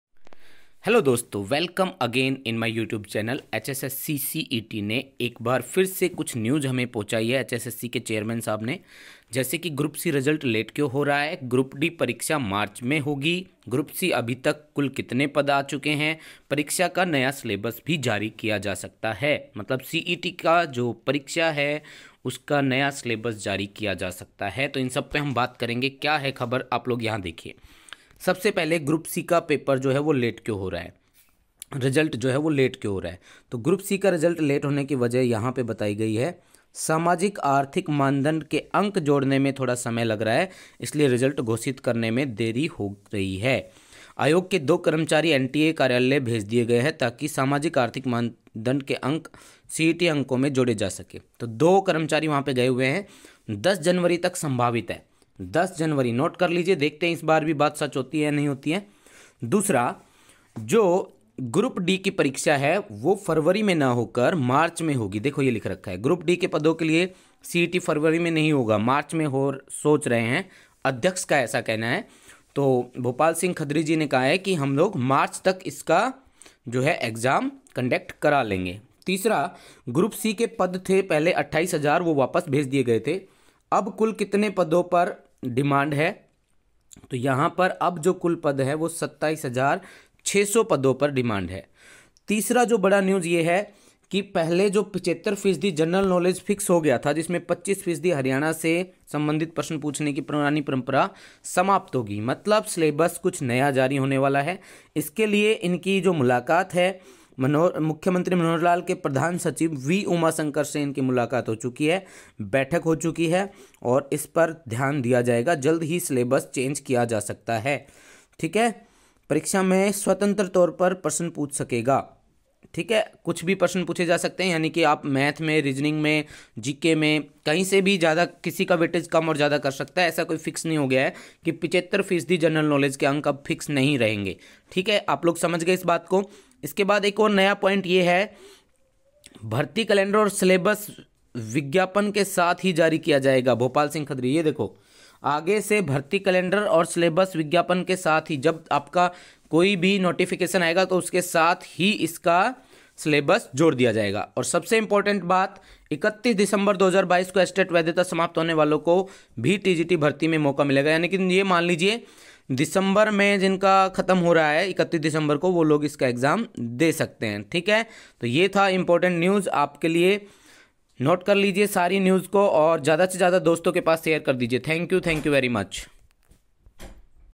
HSSC CET result Date| HSSC CET Group D Exam Soon | HSSC CET Syllabus Change
According to HSSC Chairman Bhopal Singh Khadri Group D exam will be held in March. Group C result will be declared upto 10th January. Some other news also given by chairman.